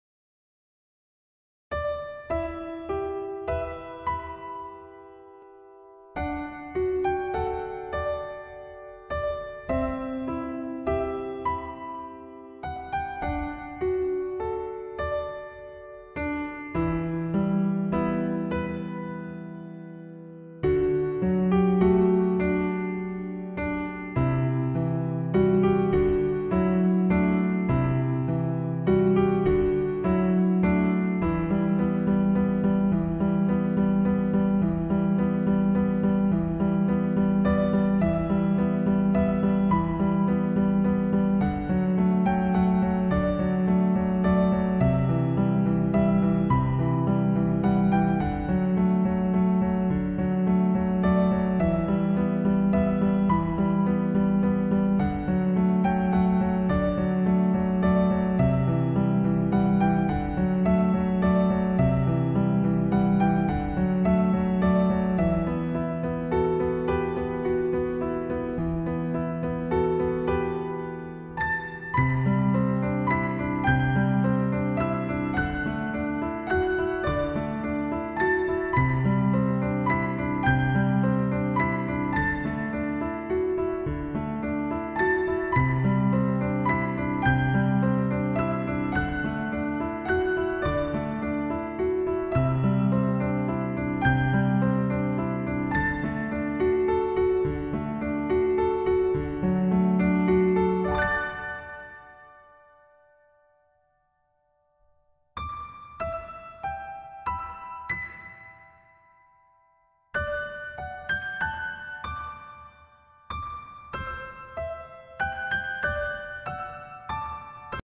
Pray for Paris Speedpaint
I pray for the one's who died and the one's who survived. I feel really bad and I cried while I drew this...

Logiciels: Paint Tool SAI
Enregistrement: Hypercam 2
Montage: Movie Maker